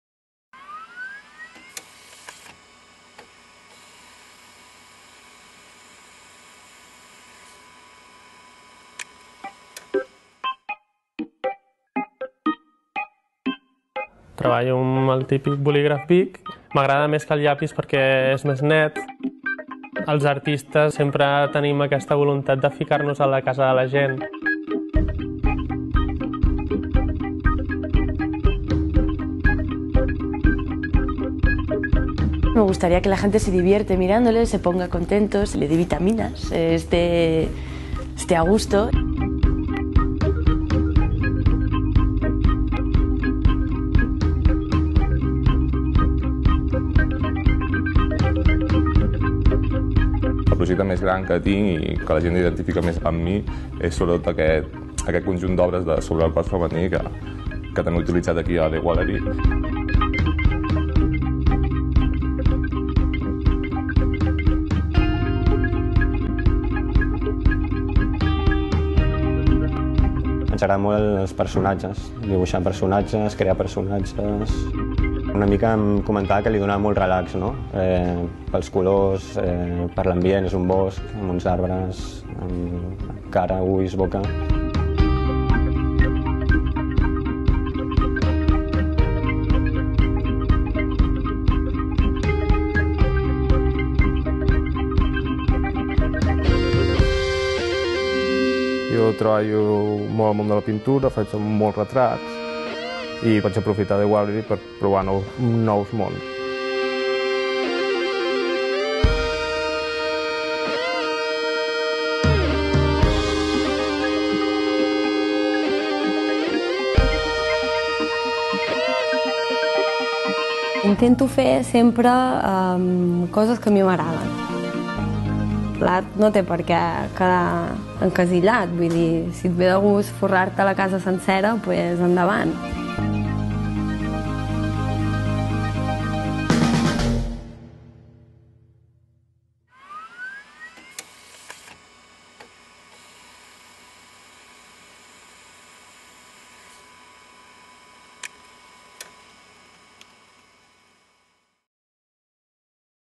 [ESP] The Wallery - ARTE SIN ENMARCAR
Somos una selección única de artistas internacionales y emergentes que creamos murales y vinilos de autor.

Nuestros Walleristas: Amaia Arrazola, BANDID8, Brosmind, Caroline Selmes, Chamo San, Conrad Roset, Guim Tio, Home de Caramel, Ivan Bravo, Maria Diamantes.

Este vídeo no hubiera sido posible sin E2S Productora ¡Gracias!